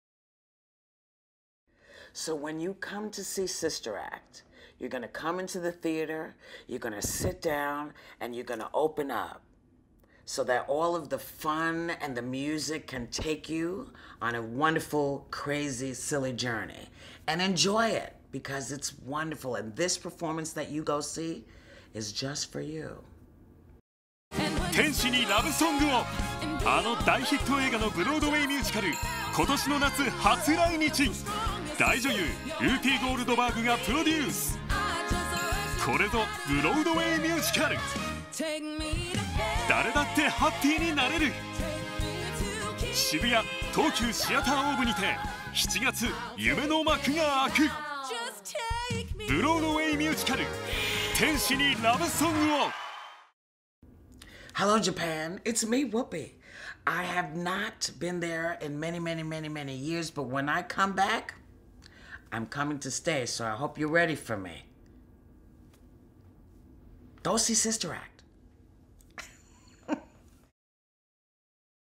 Bunkamura 東急シアターオーブ「天使にラブ・ソングを…」ハリウッド女優、ウーピー・ゴールドバーグからメッセージ到着！
ブロードウェイ・ミュージカル
天使にラブ・ソングを…（ シスター・アクト ）

映画「天使にラブ・ソングを･･･」で主人公デロリスを演じたウーピー・ゴールドバーグが、自らプロデューサーの一人として作り上げたミュージカル。
作曲は｢美女と野獣｣「アラジン」等の有名作品でアカデミー賞を８回も受賞のアラン・メンケン。　次々に繰り出される名曲に乗せて、友情と笑いに満ちたストーリーがスピーディーに展開し、観た後はすっきり爽快！元気になれる、これぞミュージカル。

2015/7/15(水)～8/2(日)
東急シアターオーブ（渋谷ヒカリエ11F）
絶賛発売中！

公演詳細は下記をご覧ください。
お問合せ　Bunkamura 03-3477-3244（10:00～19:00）